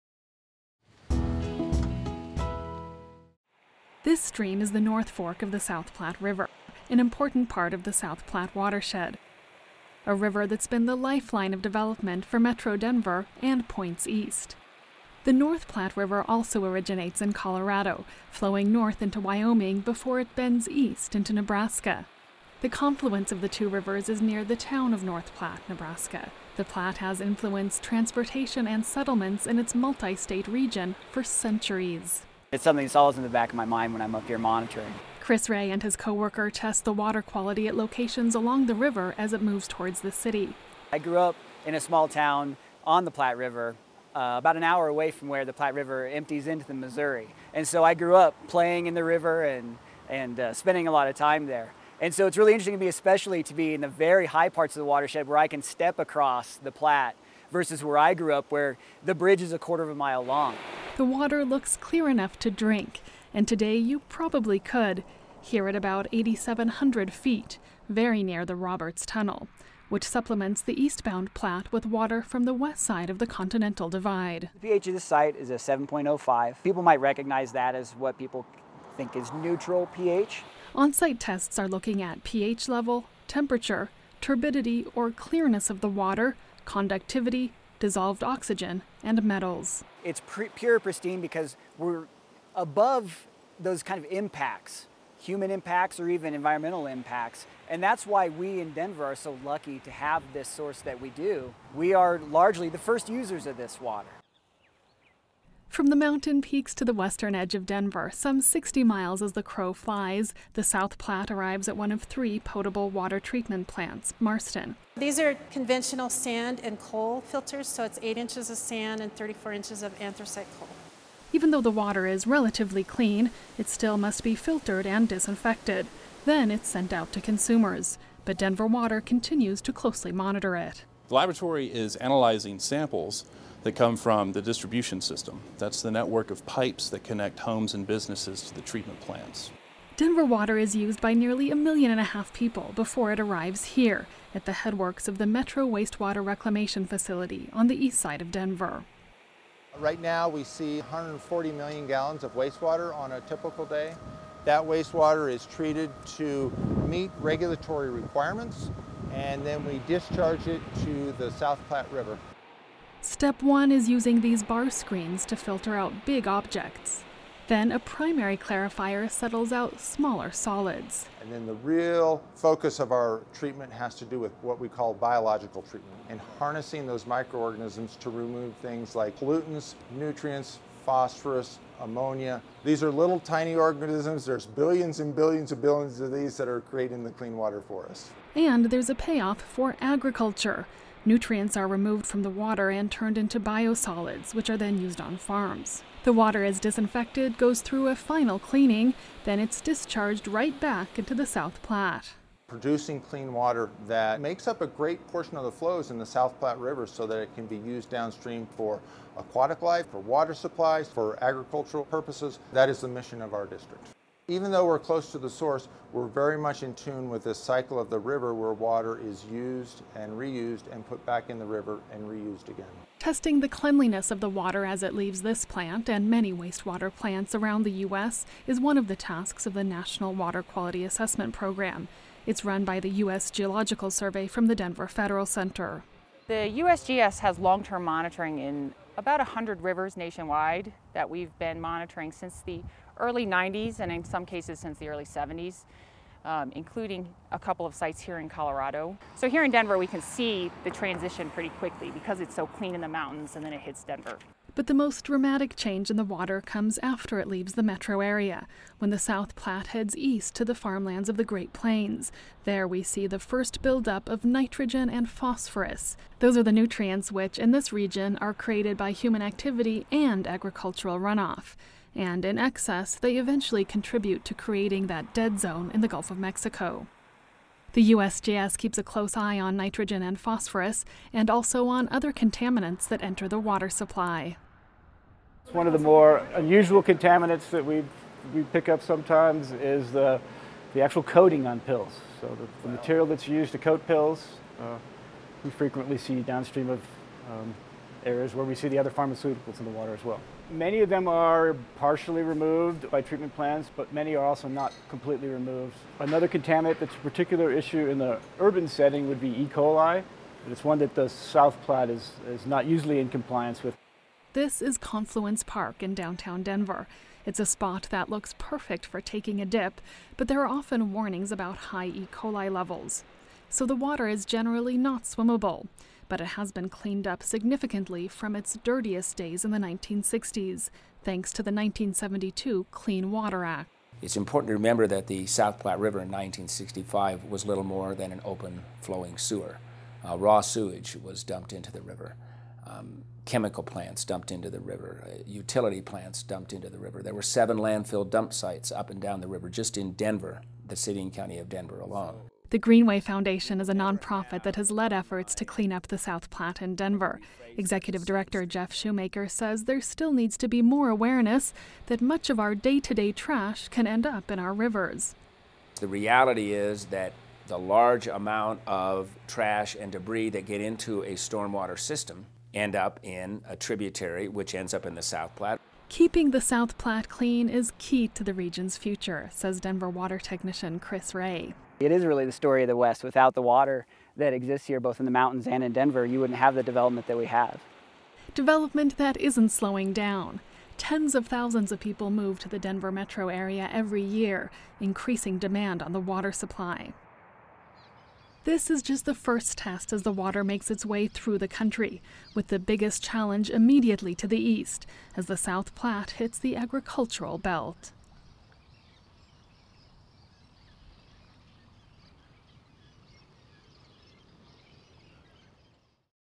Pure and Pristine and Tested
When snow melts on the eastern side of the continental divide in the Rocky Mountains, it is clean enough to drink. But very quickly, that water will need to be used by nearly 1.5 million people in the Denver Metro area. Watch what happens as water is tested and used and put back in the South Platte River to serve agriculture needs to the east.